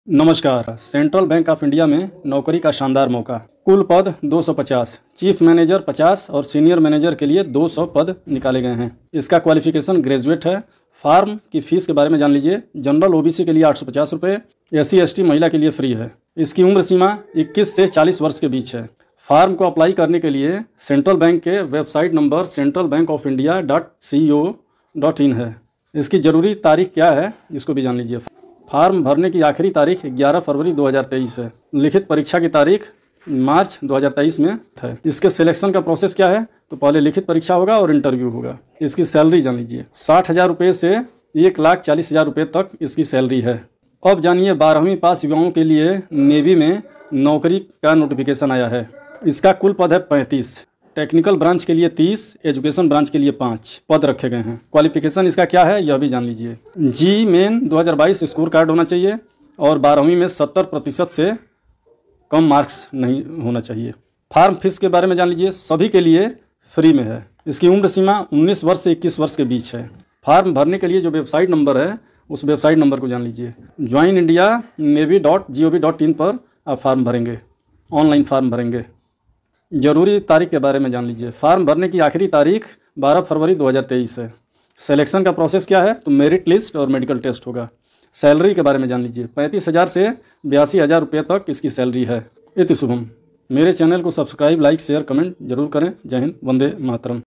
12वीं पास युवाओं को Navy में मौका । सेण्ट्रल बैंक आफ इंडिया में नौकरी निकली । Rakesh Pandey 2m ।
12वीं पास युवाओं को Navy में मौका । सेण्ट्रल बैंक आफ इंडिया में नौकरी निकली । Rakesh Pandey 2m । government jobs । How to apply navy and central bank of India vacancy online । Job Alert । Sarkari naukari ।